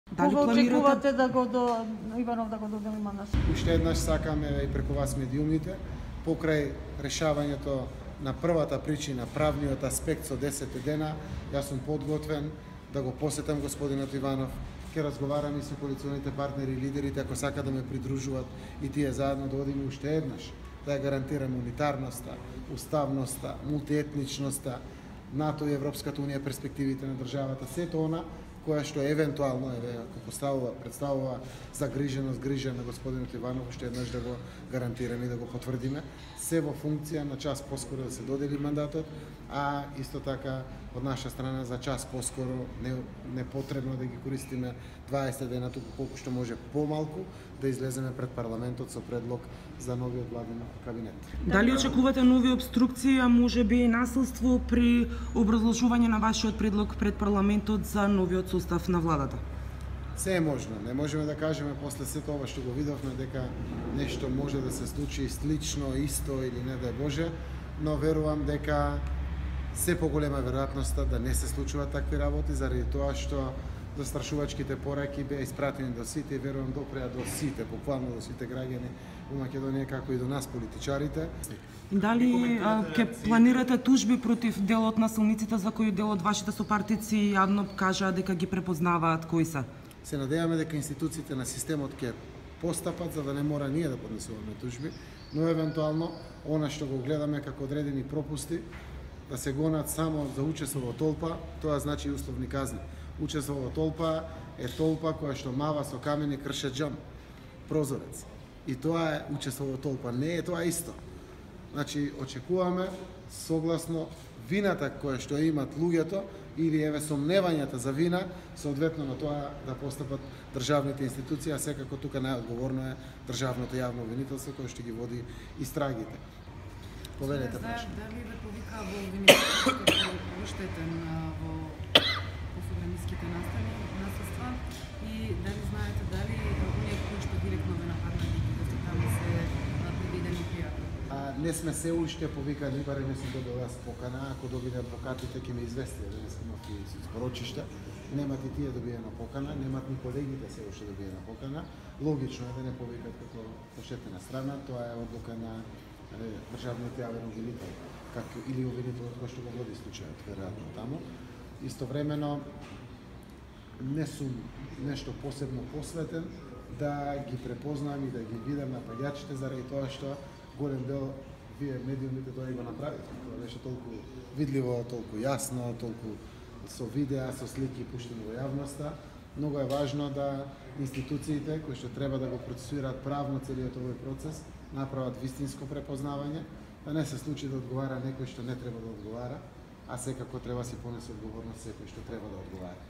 Заев го очекува мандатот од Претседателот Иванов
Претседателот на СДСМ Зоран Заев, изрази готовност да оди кај Претседателот Иванов за да даде гаранции за унитарноста на Македонија. Тој не верува во можноста од нови насилства